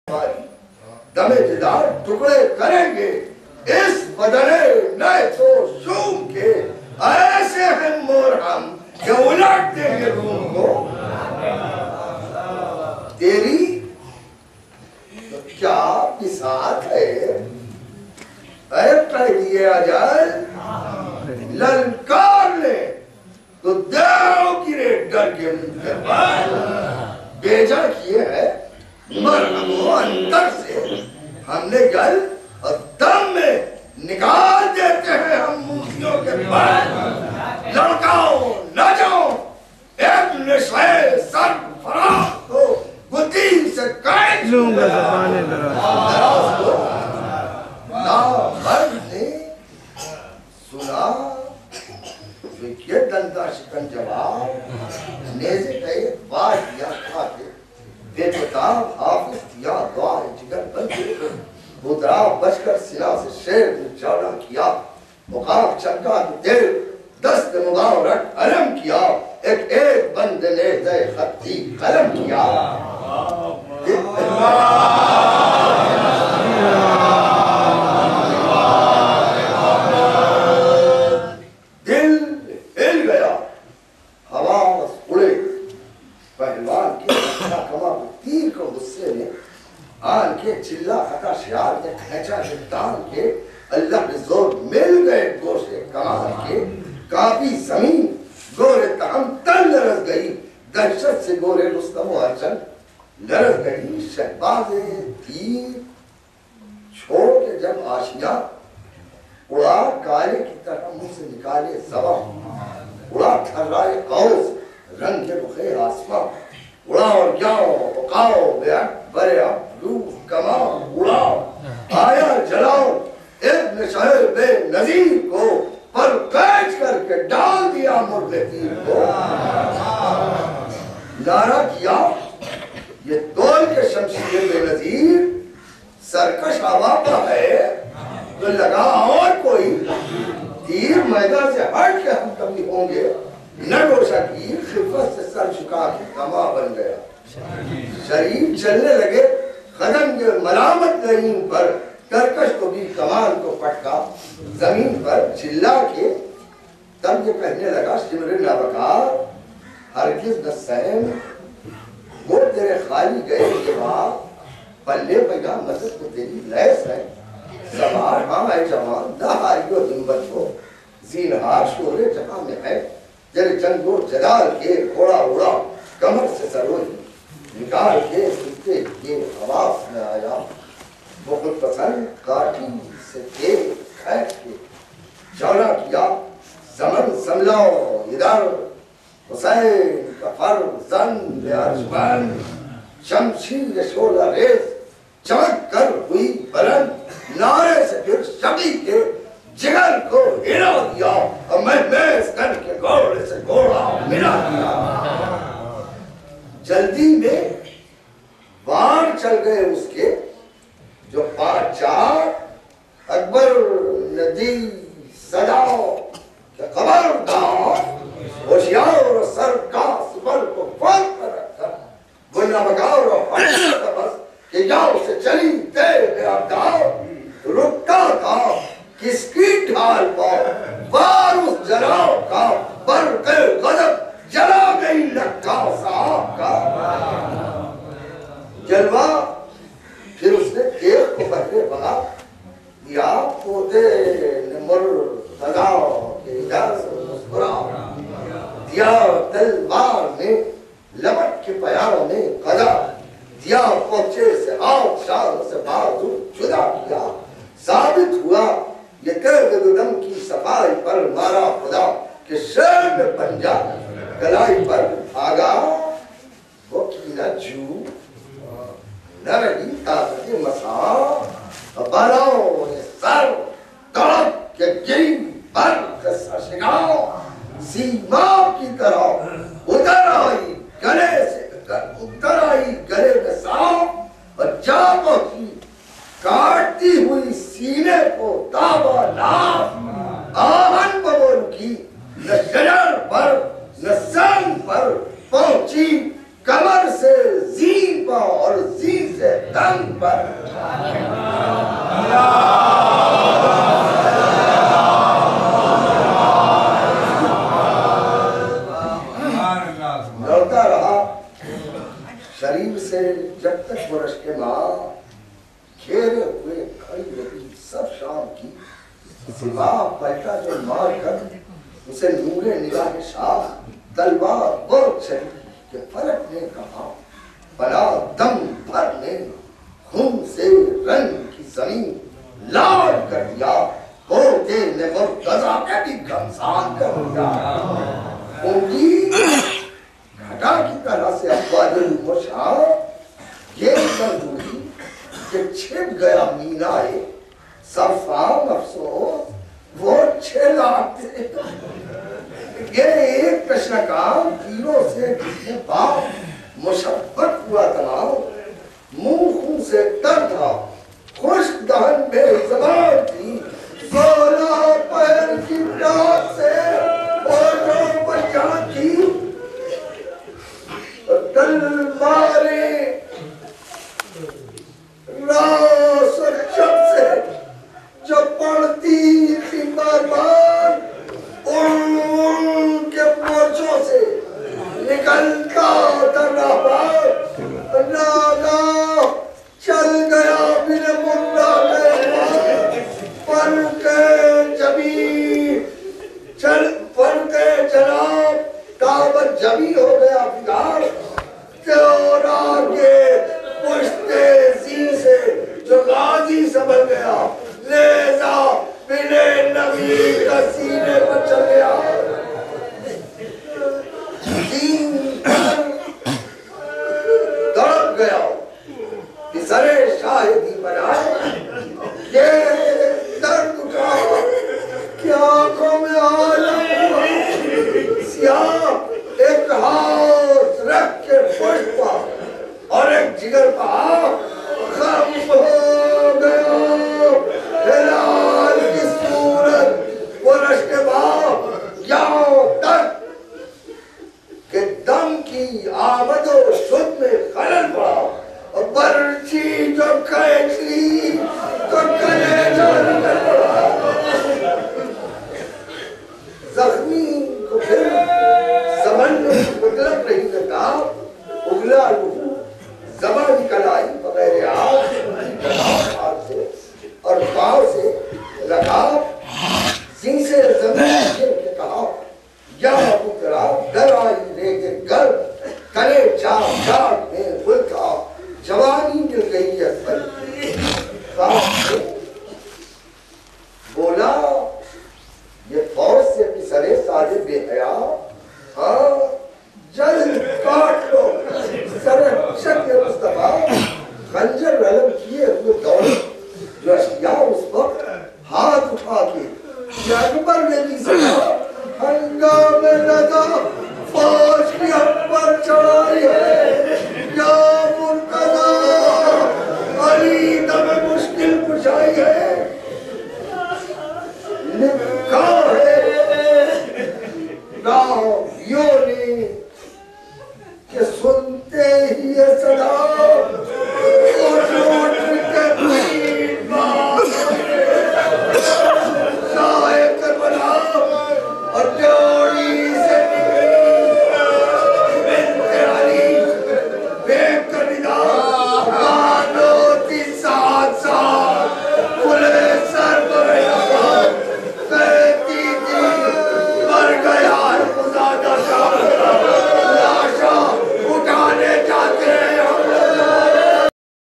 Majalis e Shabih e Taboot Hazrat Ali Akbar (A.S.) | Marsiya Khwani by Baqir Jaisi Sb (Part 3) Jais
Marsiya read on 9th muharram by Baqir Jaisi Sb 3rd part. video by shubair naqvi